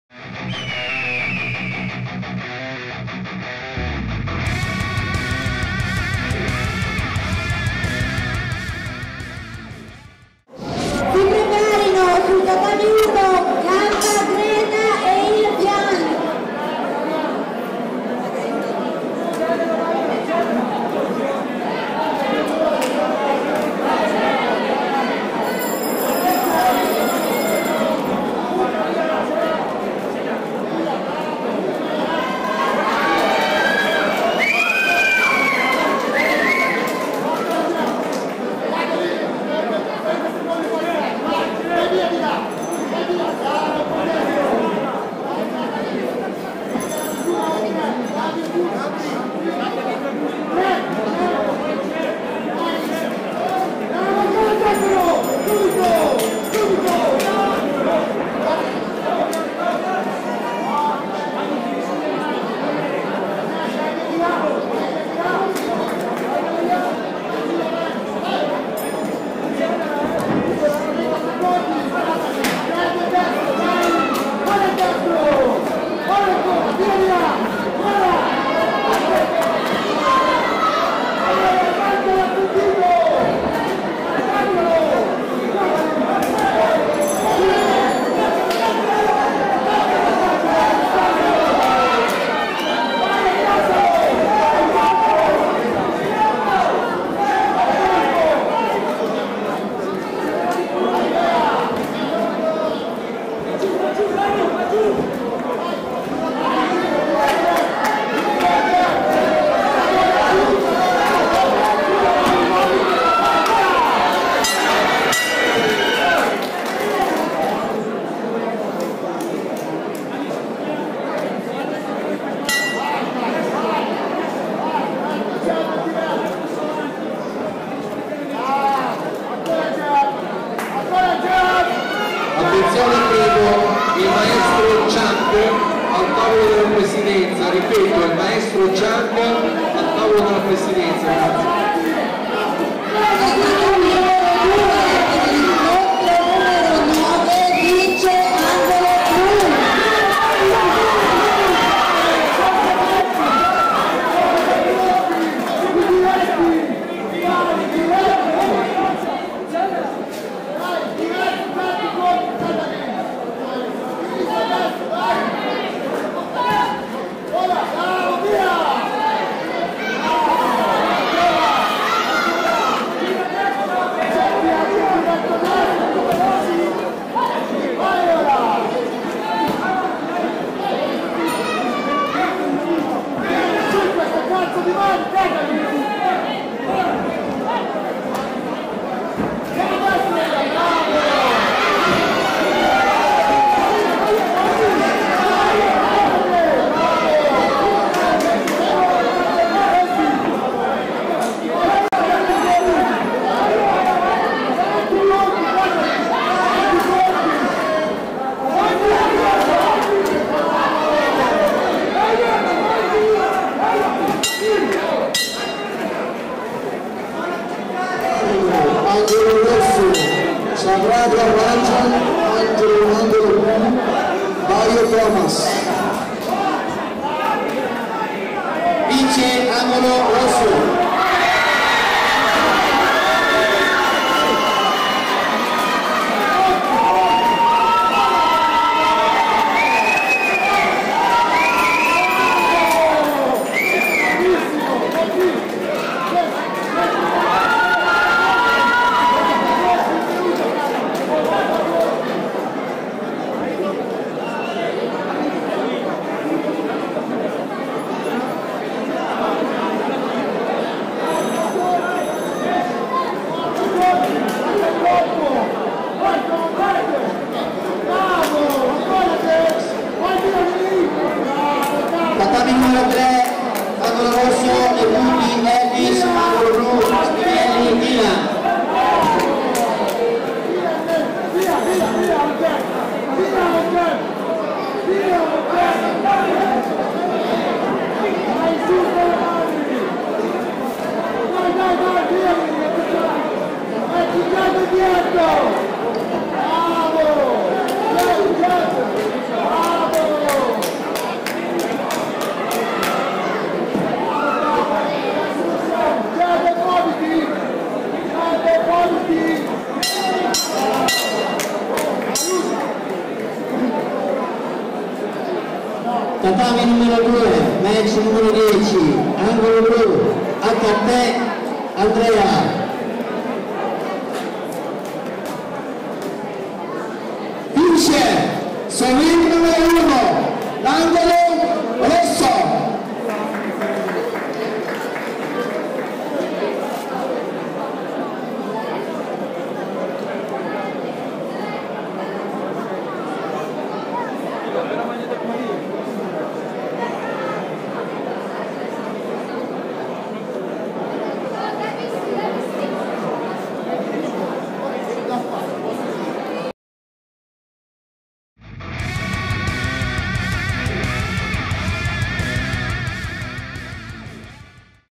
7° match Ring 1 – Bottazzi Catalin VS Grillo Daniele International CUP WBFC 2022
International CUP WBFC
27 Febbraio 2022

Full Contact / Contatto Pieno

Si ringrazia:

Montaggio video: Carlos Ross e Simone Perlini